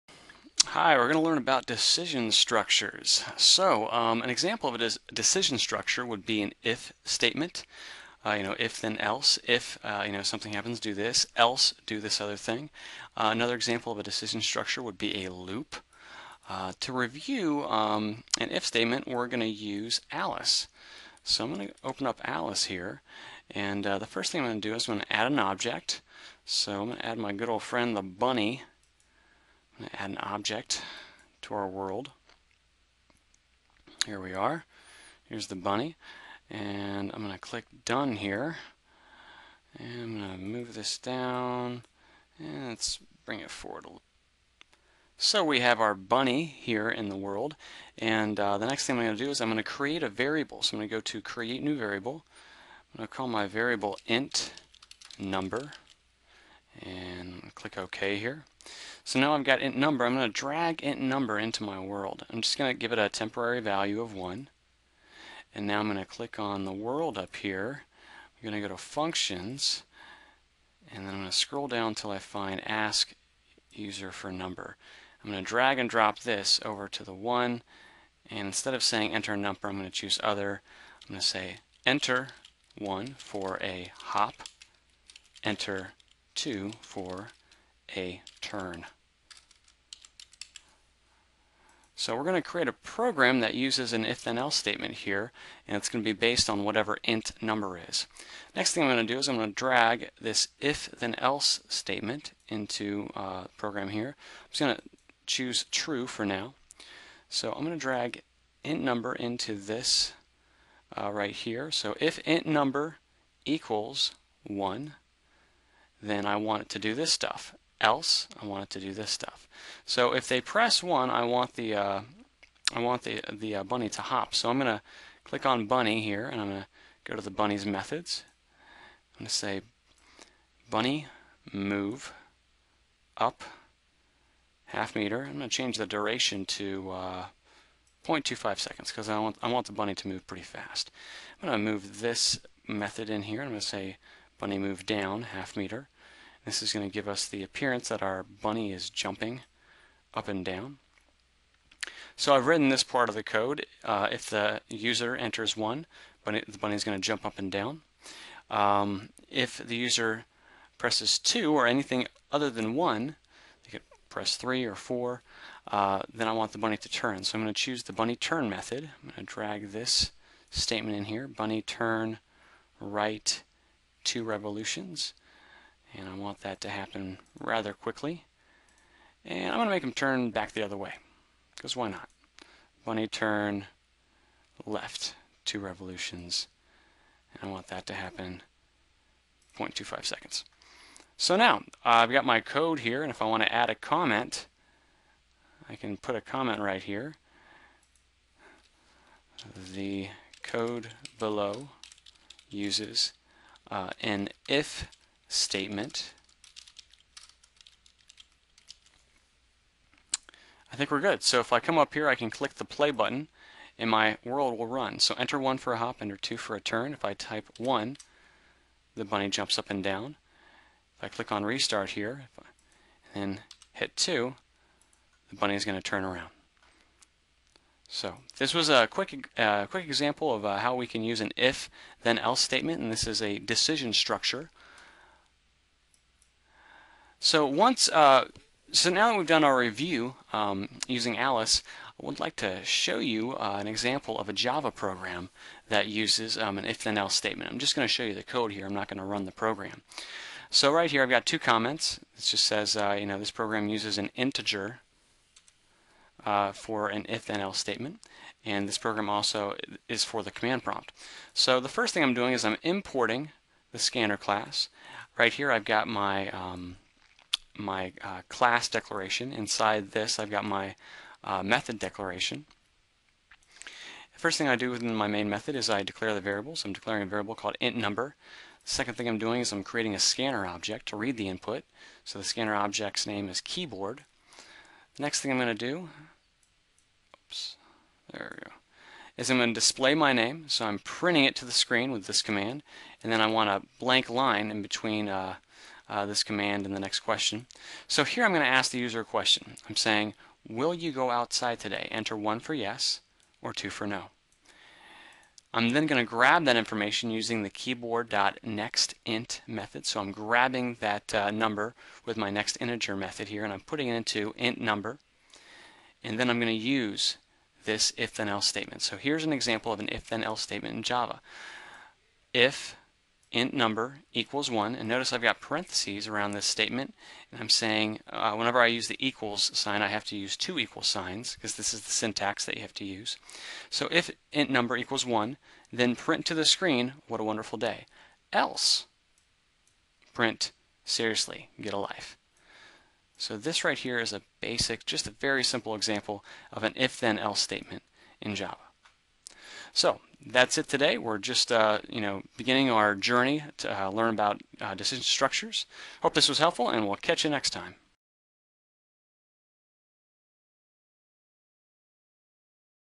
026 - Decision Structures Intro
This brief tutorial introduces the concept of decision structures in programming.  For a complete list of ordered lessons, please